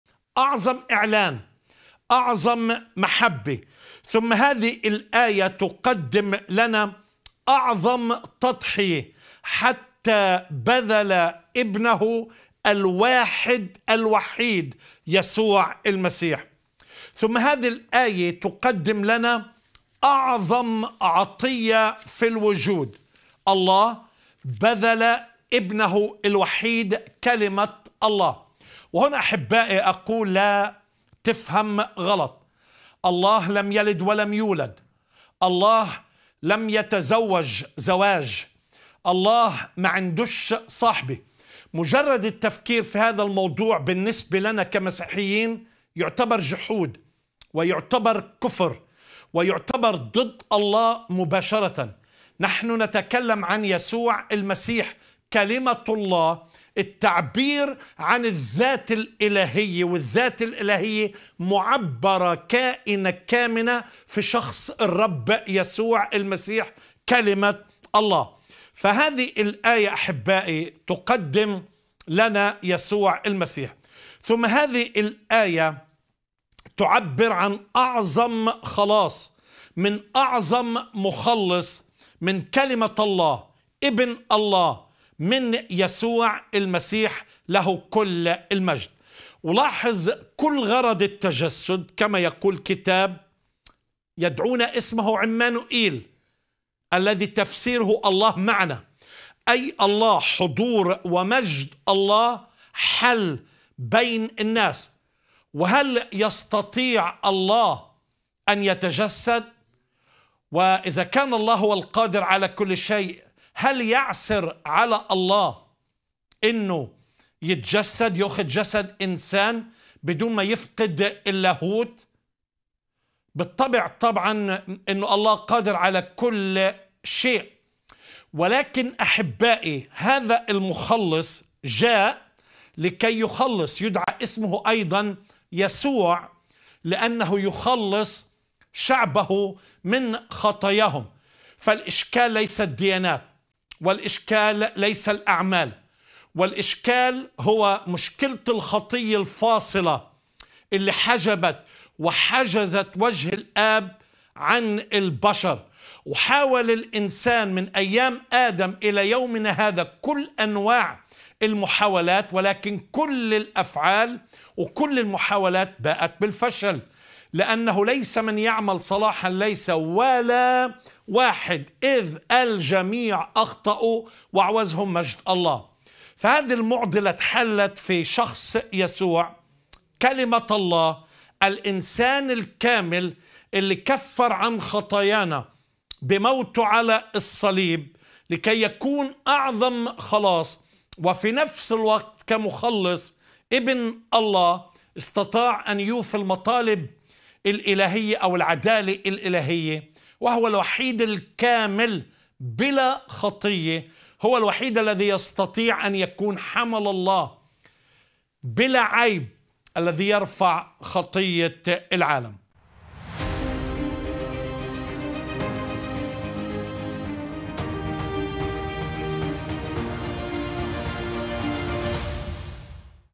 03- ما معنى أن المسيح هو ابن الله
ماذا تعني بنوة المسيح لله ؟
وهل يؤمن المسيحي بإله يلد ويولد؟

🔖جزء 3 من حلقة " لأنه هكذا أحب الله العالم"
🔗 لمتابعة الحلقة كاملة 👇

📌يمكنك التواصل معنا بصورة شخصية عن طريق رسائل الواتساب👇
WhatsApp الواتساب:    🆆🅷🅰🆃🆂🅰🅿🅿